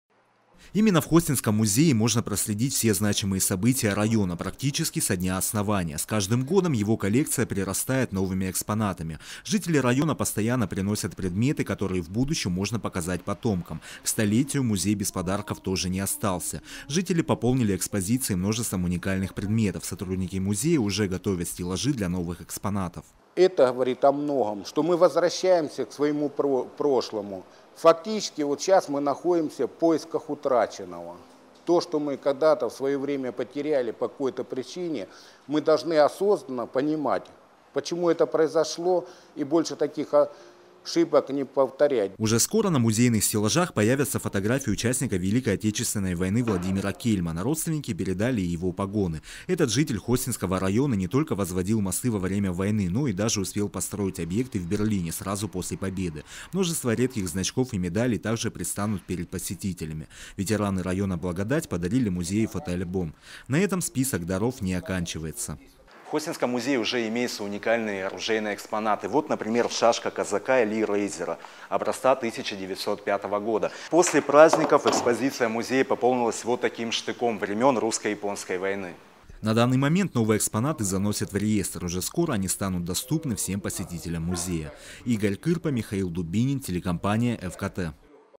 Хостинский музей отмечает 100-летие. Новости 24 Сочи
Хостинский район празднует 115-летие со дня основания. Но это не единственная памятная дата для местных жителей. В этом году и Хостинский музей отметил круглую дату — 100 летний юбилей.
Сочи. Новости.
Телеканал Эфкате.